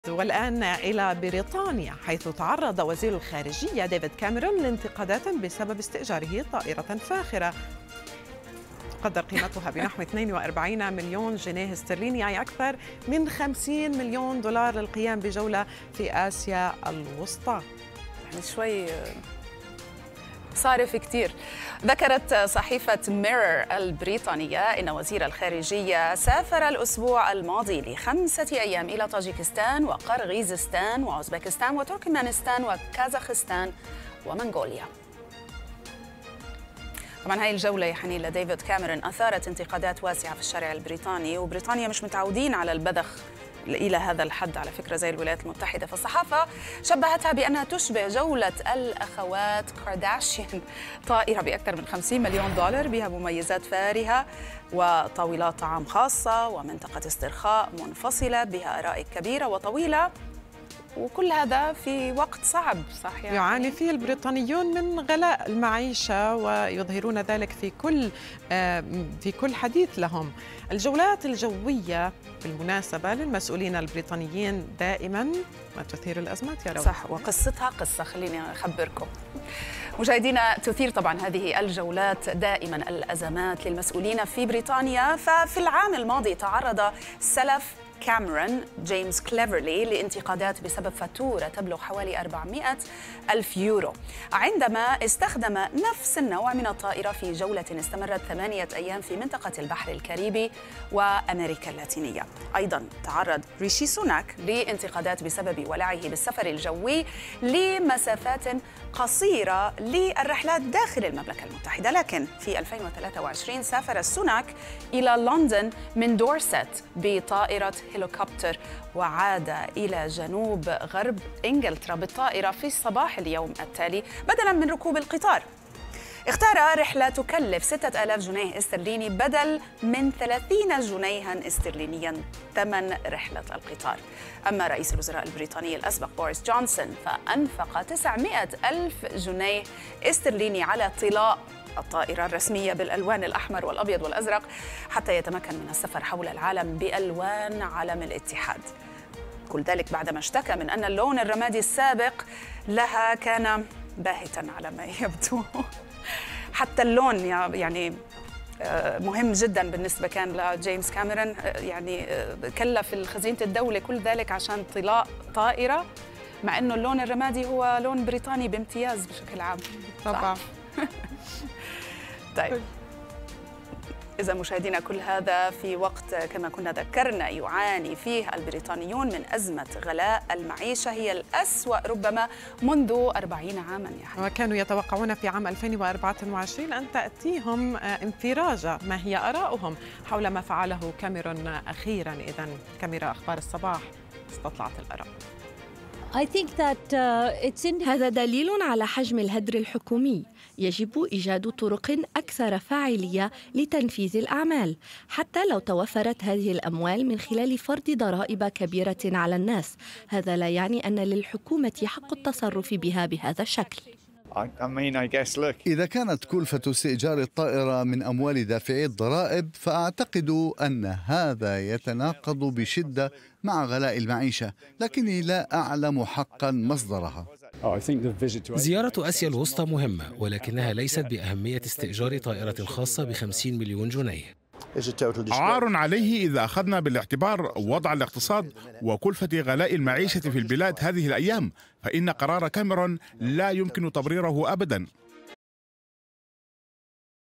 انتقادات لكاميرون لاستئجاره طائرة فاخرة للسفر.. كم تبلغ الكلفة؟
أخبار_الصباح | انتقادات لـ كاميرون لاستئجاره طائرة فاخرة للسفر.. كم تبلغ الكلفة؟
لمعرفة أحدث المستجدات.. عبر البث المباشر من هنا ⤵
وبإمكانكم متابعة العربية على منصات التواصل